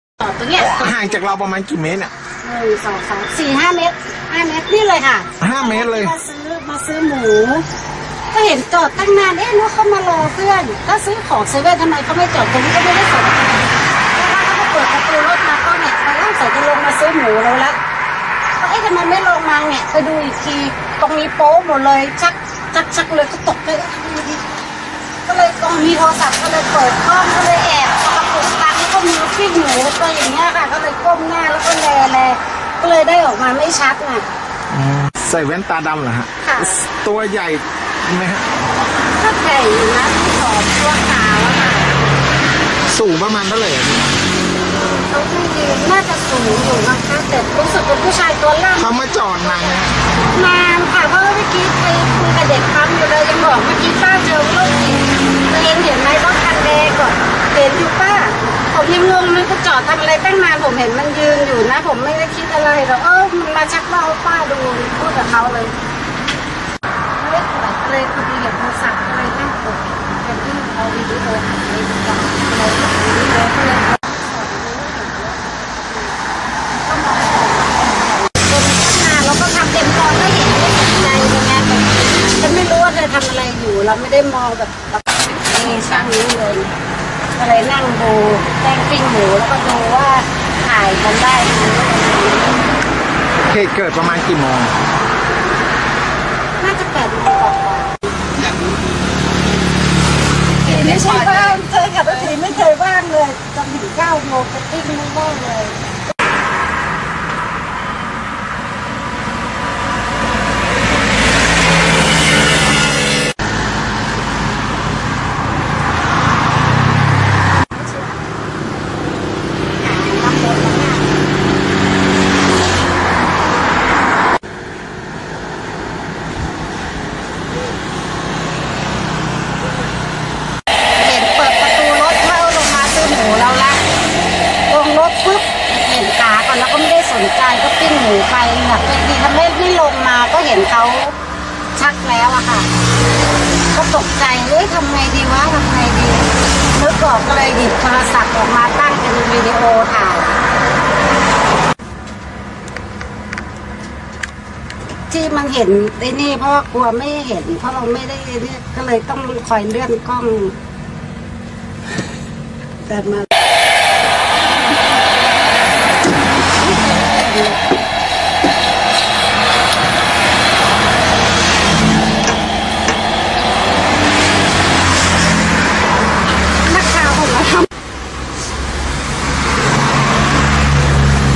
เตือนภัย!ไอ้โรคจิตจอดเก๋งสำเร็จความใคร่โชว์แม่ค้าหมูปิ้งสาวลำปางกลางวันแสกๆ
06/10/2018 เตือนภัย!ไอ้โรคจิตจอดเก๋งสำเร็จความใคร่โชว์แม่ค้าหมูปิ้งสาวลำปางกลางวันแสกๆ
ลำปาง – แม่ค้าหมูปิ้งสาวลำปาง โพสต์คลิปเตือนภัย..หลังพบไอ้โรคจิตขับเก๋งจอดใกล้ร้าน เปิดประตู-ถอดกางเกงสำเร็จความใคร่ต่อหน้าต่อตากลางวันแสกๆ